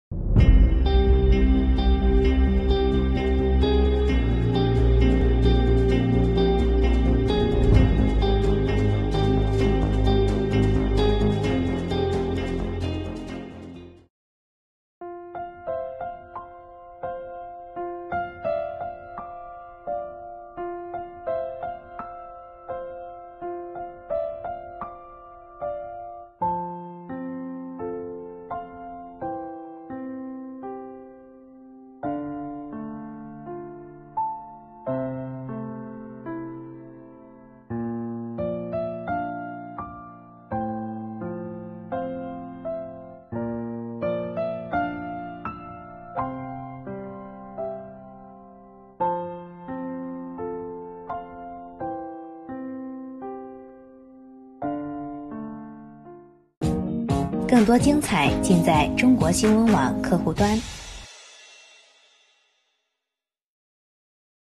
感人！中国福建一轿车司机操作失误连撞3车 众人抬车救人
17日，福建晋江闹市路口一宝马车连撞电动车和2辆小车，骑手被卡动弹不得，但接下来短短几秒钟发生的一幕，却感动了所有人。